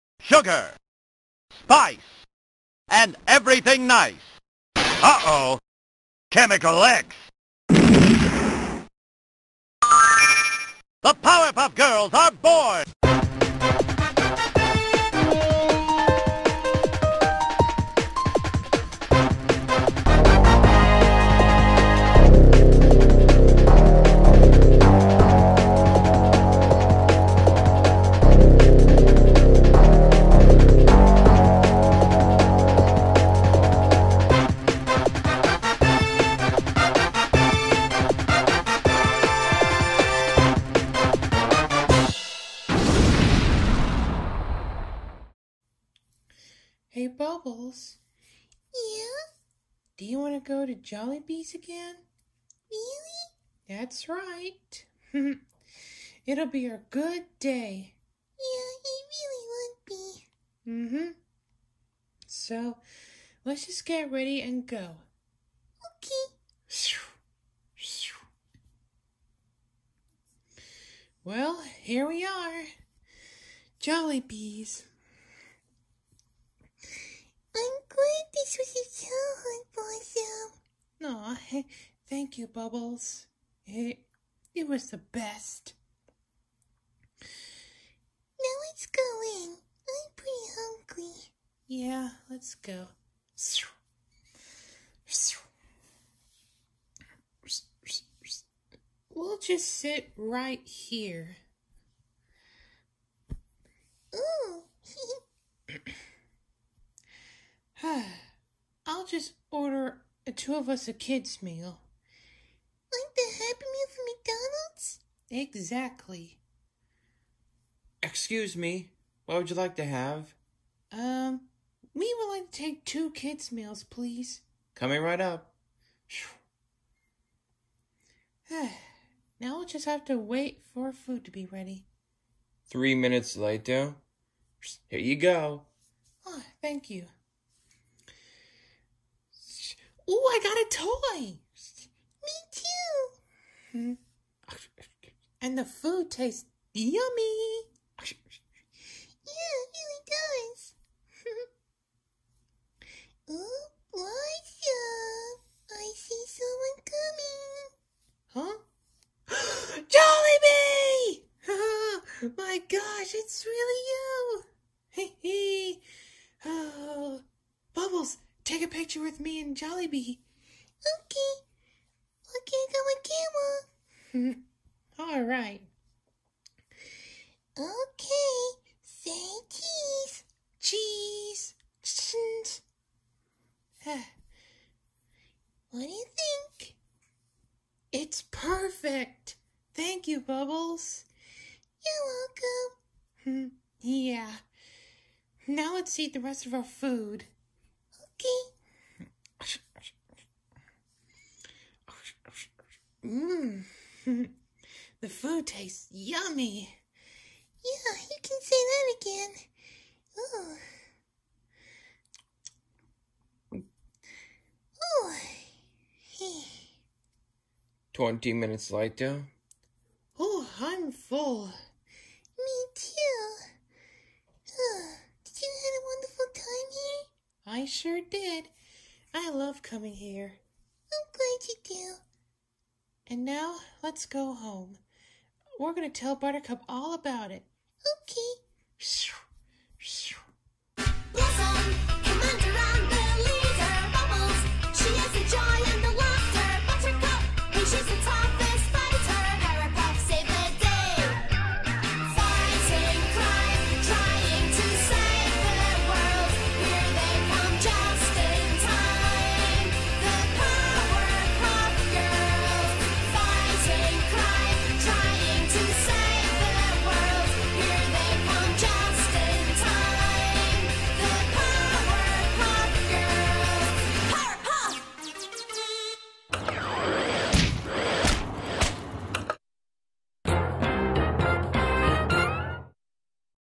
Powerpuff Girls:Visiting Jolibees Again
Blossom and Bubbles decided to visit Jolibee’s restaurant again and they ordered a kids meal and then Jolibee himself showed up and Bubbles took a picture after that they ate their food and flew home to tell Buttercup